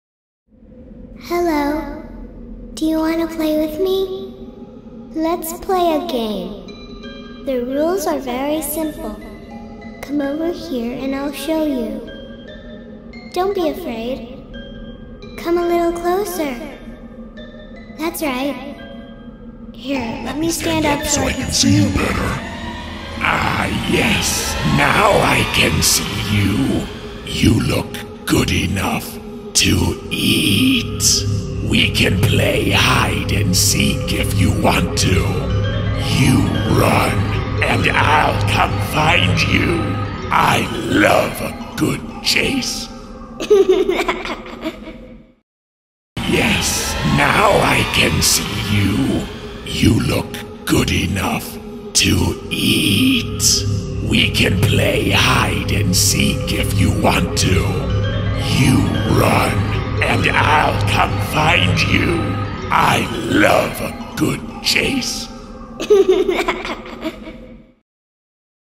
Rising Creeper MR124325
Animated Creeper Character has a soft PVC head with a moving mouth and light-up eyes, a tattered costume with gauze details and blow-molded hands. The prop starts at a 40" height with the head down and the arms folded into the  chest; as he rises to 60" the head lifts, eyes light up and arms open out to the side. Sturdy metal frame has the rising mechanism attached.
Also includes a UL adapter, IR sensor with Step Here pad
option included. Functions include: Steady-On, Try-Me, or Sensor. This item could also be used
with a fog machine to give a creepy effect (not included).
Recommended for indoor or covered porch use only.
When activated, he says:
"Hello...Do you want to play with me? Let's play a game!
The rules are very simple...come over here and I'll show you...Don't be afraid. Come a little closer...that's right... Here, let me stand up so I can see you better... (His voice changes) Ah yes, now I can see you--you look good enough to eat! We can play hide and seek if you want to...you run... and I'll come and find you! I love a good chase! (Gives a childish giggle)"